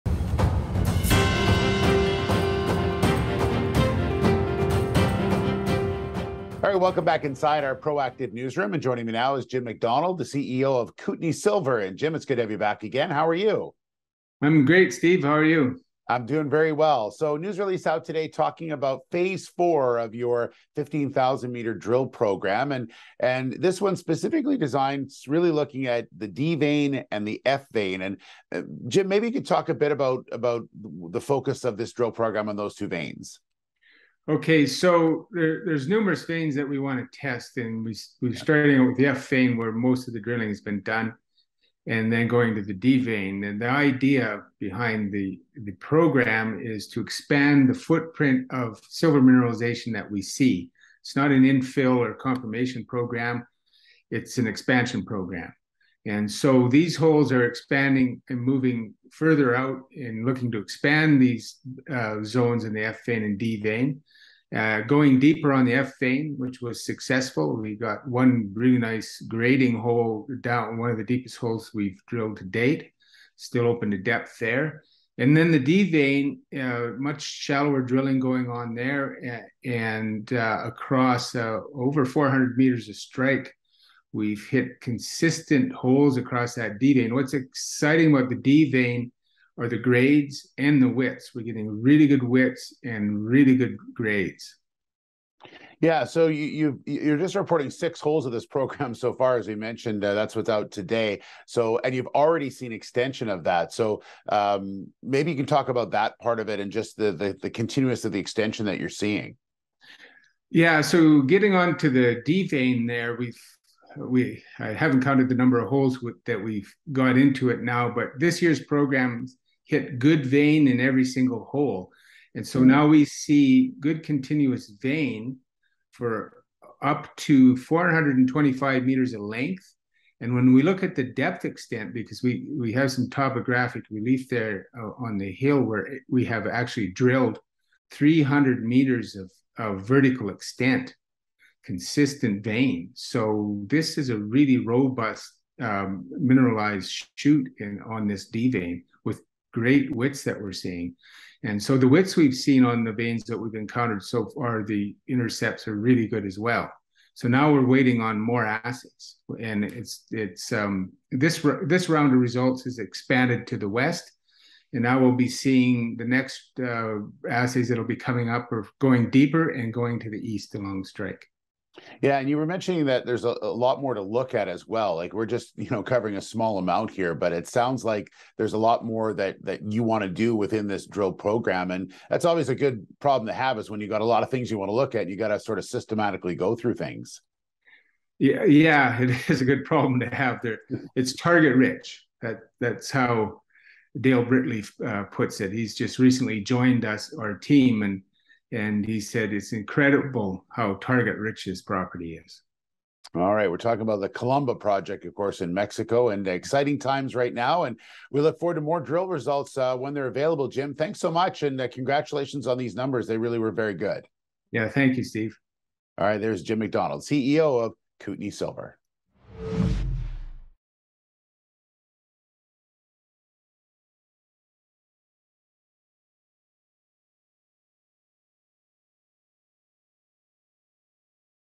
Kootenay Silver announces positive drill results from Phase 4 program at Columba Project in Mexico
Kootenay Silver CEO Jim McDonald joined Steve Darling from Proactive to share the latest assay results from drilling at the Columba Silver project in Mexico.
McDonald telling Proactive these first 6 holes of the Phase 4 program have extended the known mineralization at both the D Vein and F Vein targets. The highest grade intercept saw 795 gpt silver, 0.95% lead and 1.47% zinc over one meter. The total Phase 4 program will consist of 15,000 metres of drilling.